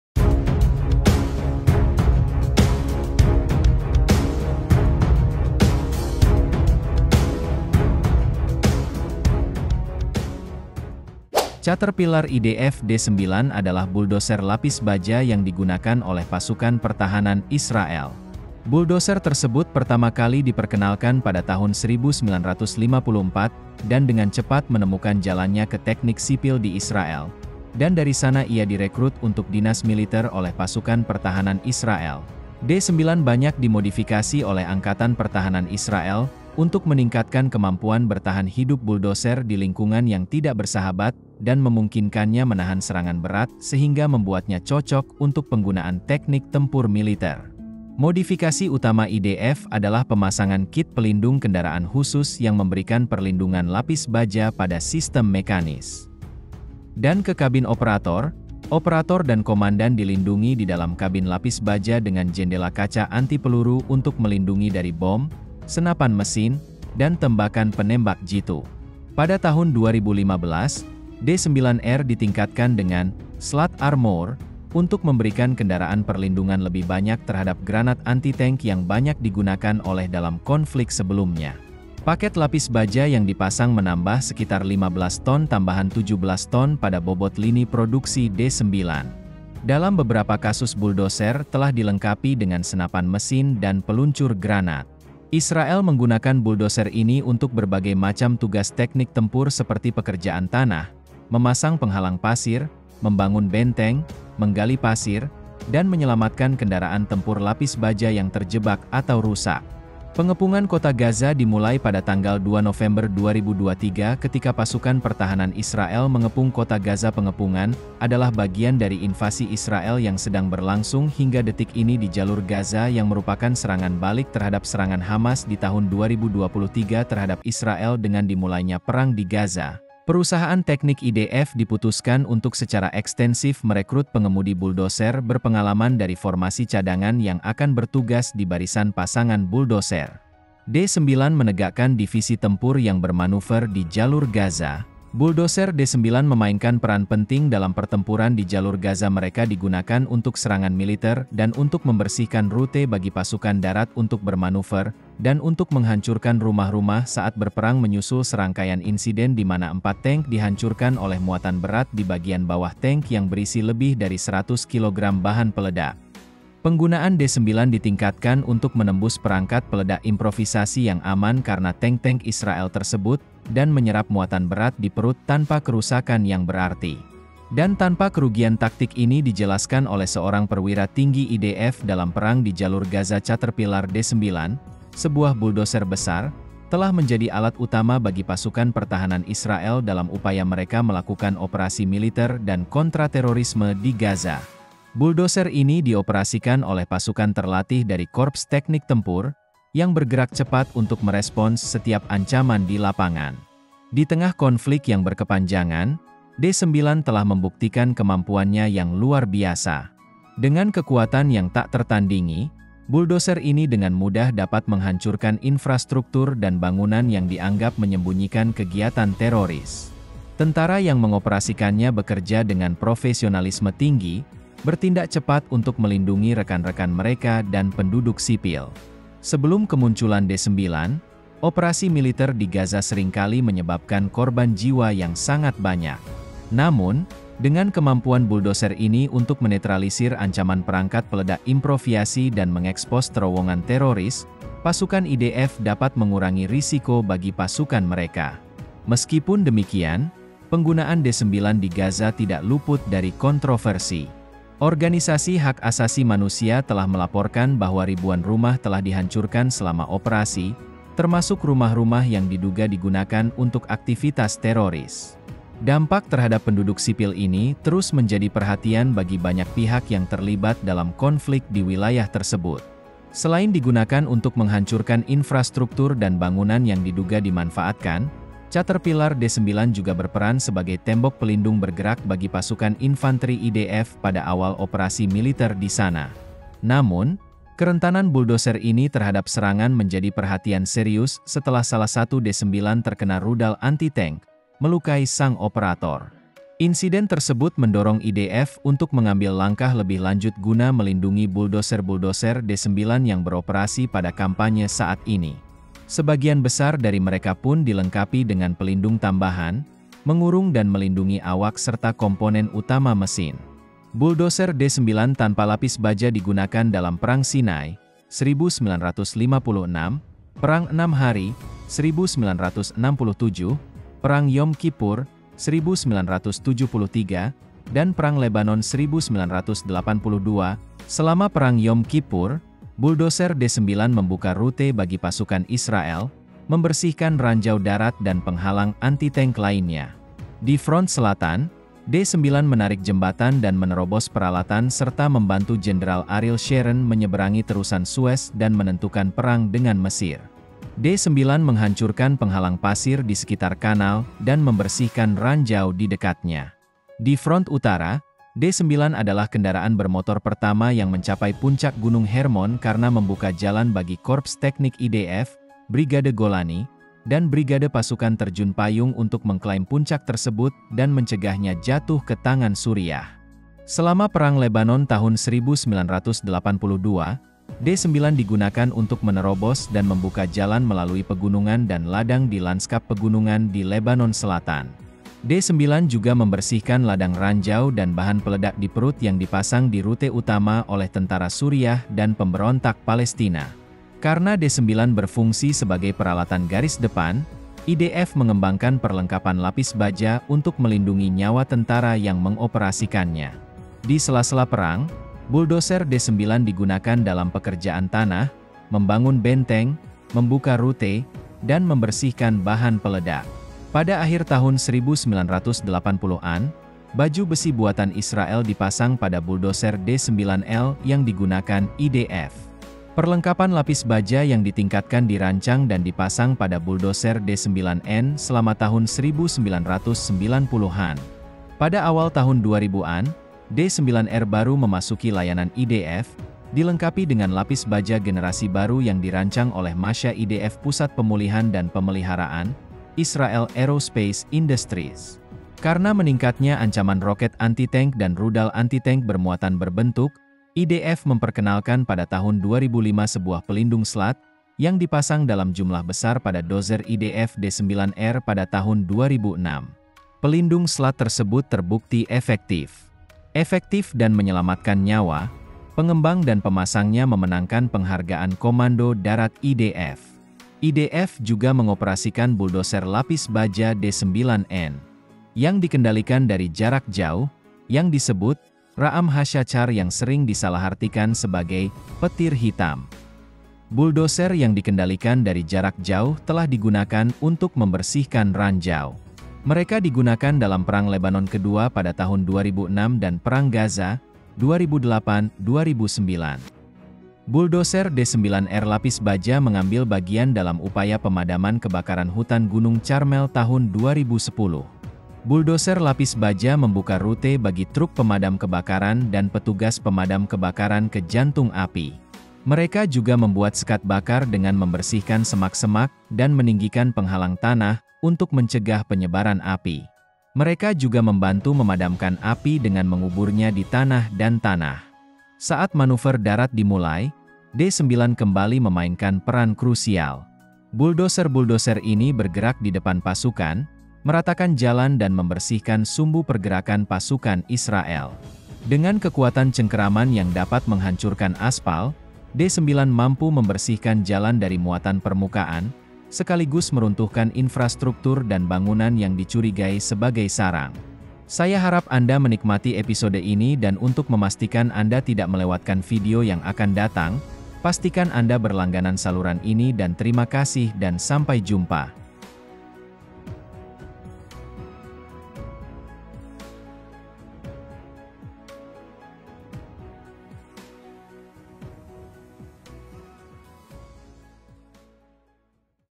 ISRAEL! KERAHKAN KENDARAAN PERANG RAKSASA YANG SULIT DIHANCURKAN
Caterpillar IDF D9 adalah buldoser lapis baja yang digunakan oleh Pasukan Pertahanan Israel. Buldoser tersebut pertama kali diperkenalkan pada tahun 1954 dan dengan cepat menemukan jalannya ke Teknik sipil di Israel, dan dari sana ia direkrut untuk dinas militer oleh Pasukan Pertahanan Israel, D9 banyak dimodifikasi oleh Angkatan pertahanan israel untuk meningkatkan kemampuan bertahan hidup buldoser di lingkungan yang tidak bersahabat dan memungkinkannya menahan serangan berat sehingga membuatnya cocok untuk penggunaan teknik tempur militer.  Modifikasi utama IDF adalah pemasangan kit pelindung kendaraan khusus yang memberikan perlindungan lapis baja pada sistem mekanis